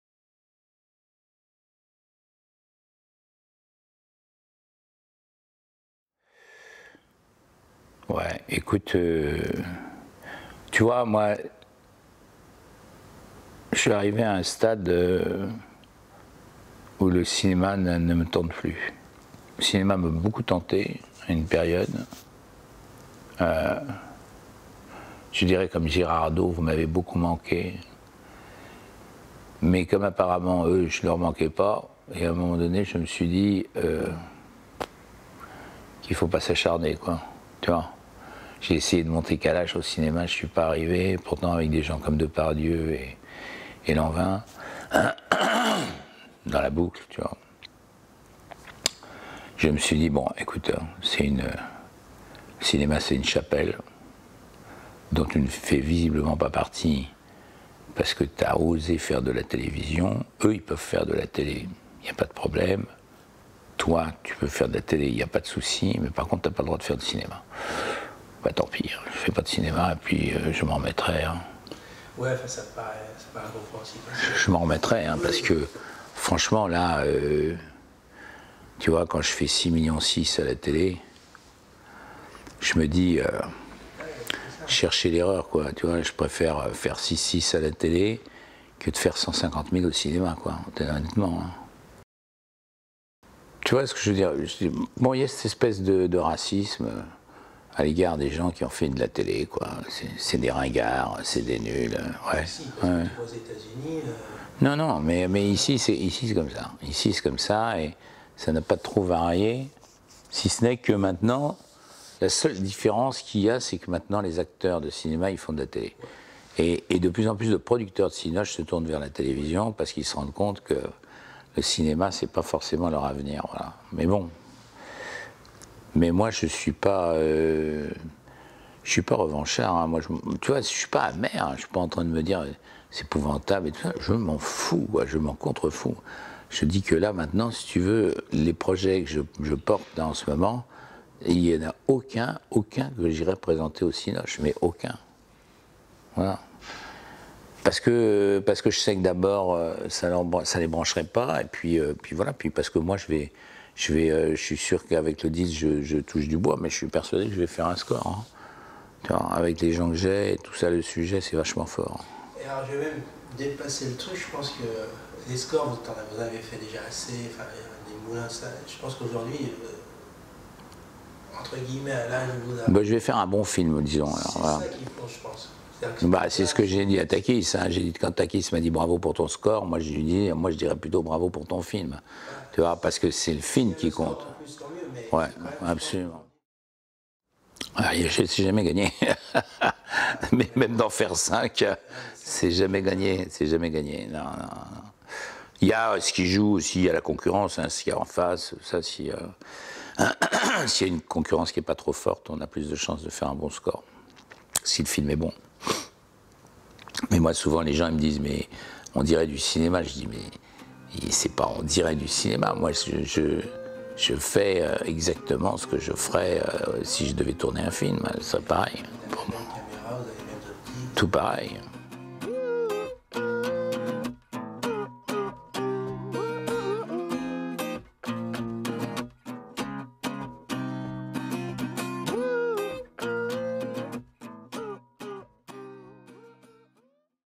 Yves Rénier "Cinéma"
Entretien avec Yves Rénier concernant le cinéma.
Réal : B.Jeanson-Béteille